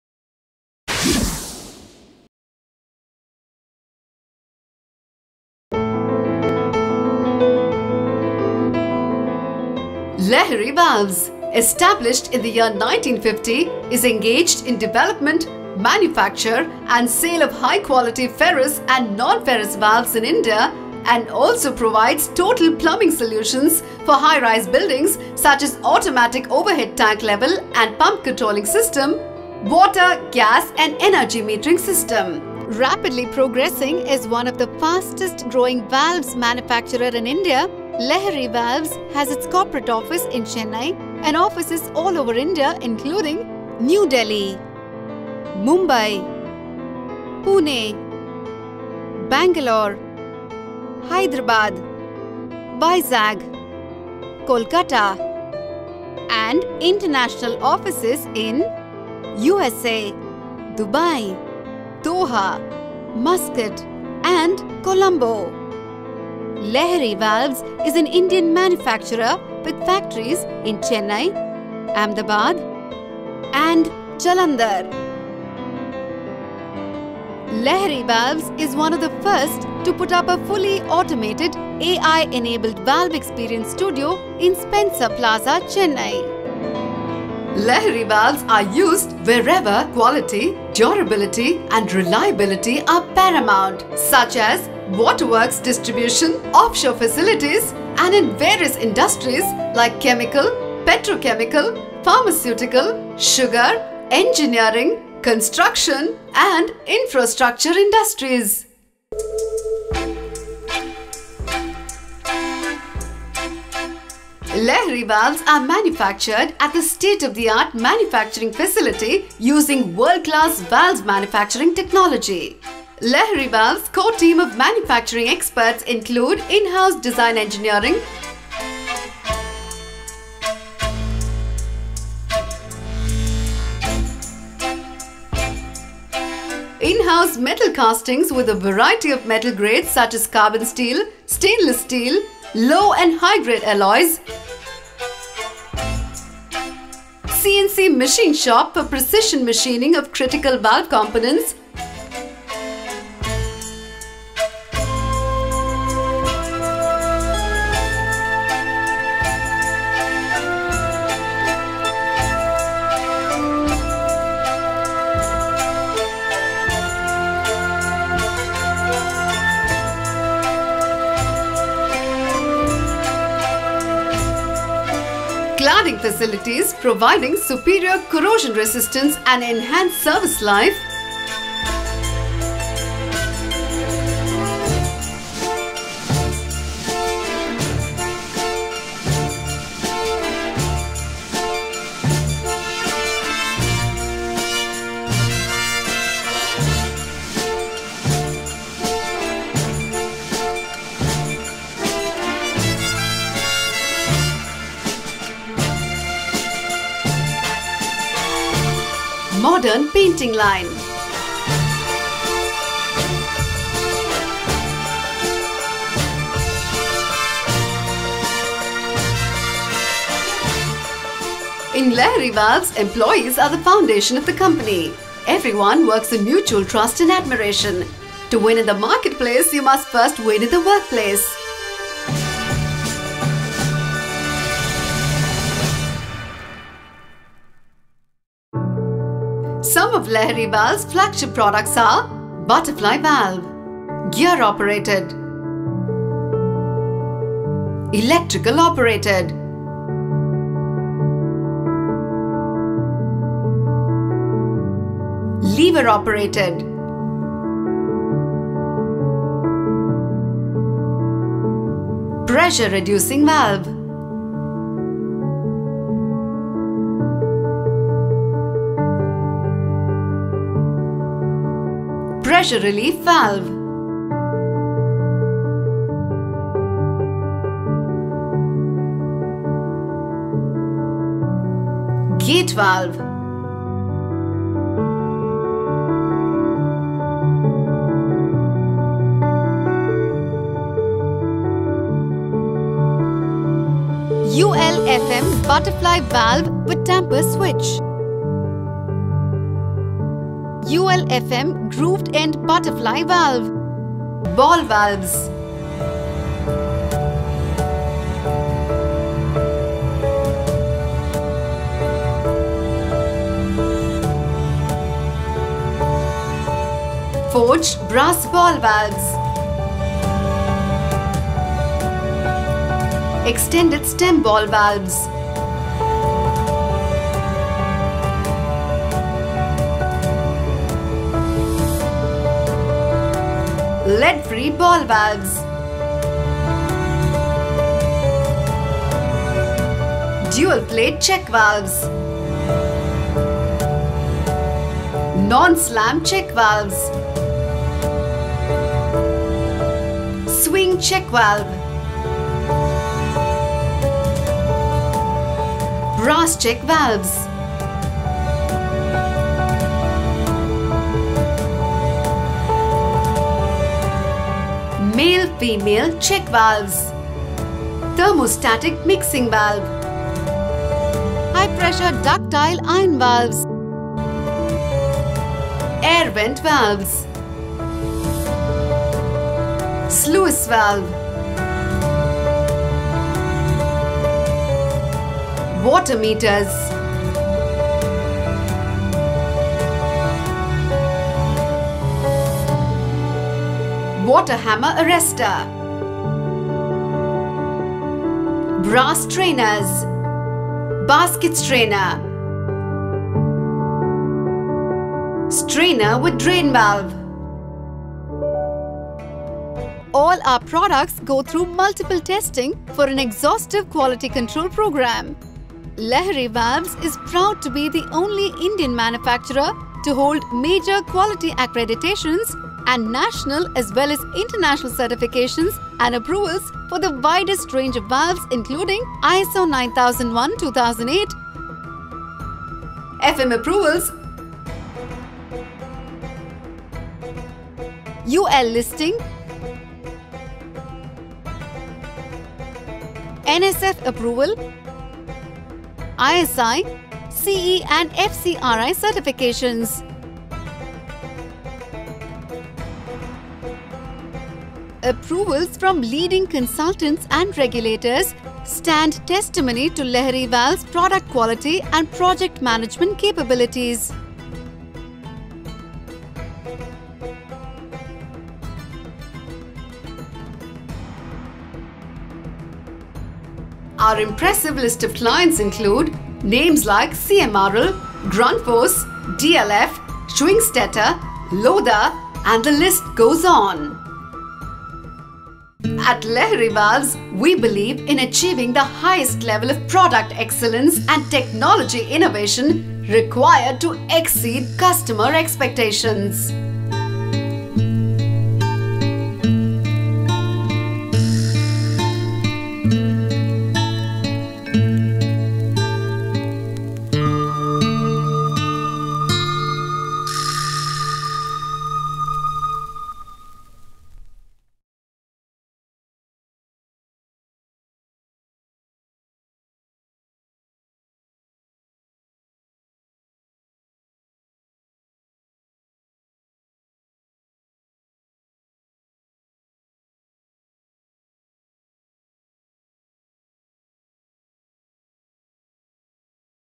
Lehry Valves - Corporate Video
Lehry Valves is a “Make in India” manufacturing company with 70 years of Industrial experience, with offices in all major Metros across India with overseas offices in Sri Lanka, UAE, and USA.

Lehry is a manufacturer and solution provider of Mechanical valves and Automated Utility management Systems for MEP services in the water services field. We cater to Industries right from Food production Pharmaceuticals, Automotive, Aeronautical, and many more.

Lehry specializes in manufacturing green and sustainable valves best suited for drinking water applications, manufacture high grade resilient valves for Firefighting, and provide water conservation solutions through automation.

We boast approvals from Local Government bodies such as BIS & ISI, international approvals/certifications such as FM, UL, NSF, and CE.